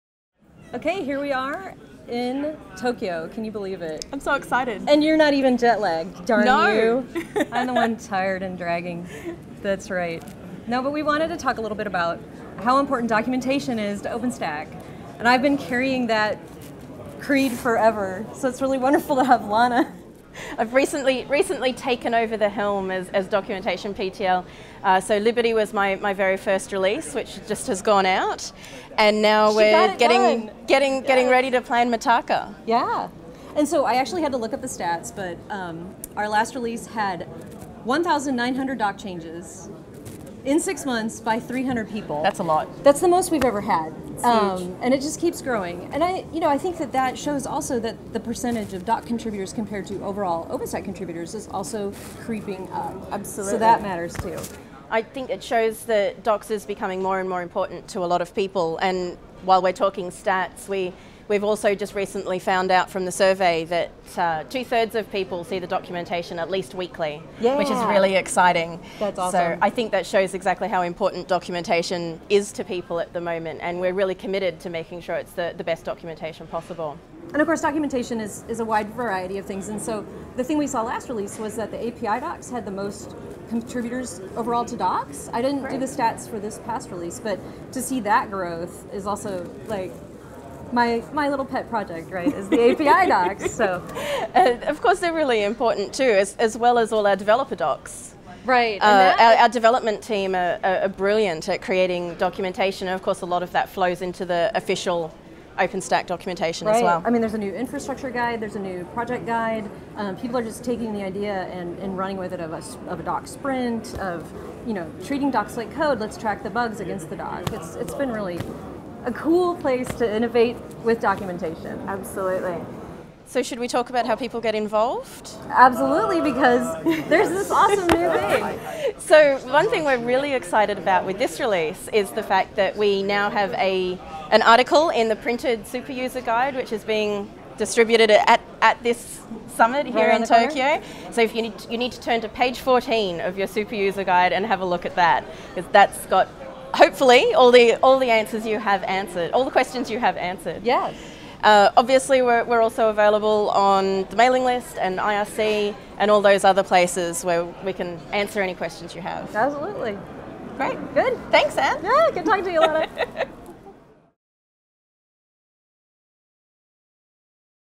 Superuser TV 2015: OpenStack Documentation
Anne Gentle, principal engineer at Rackspace and Lana Brindley, OpenStack Documentation PTL sat down with Superuser TV to discuss why Docs is important for the user community, the latest metrics from the Liberty release and how you can get involved.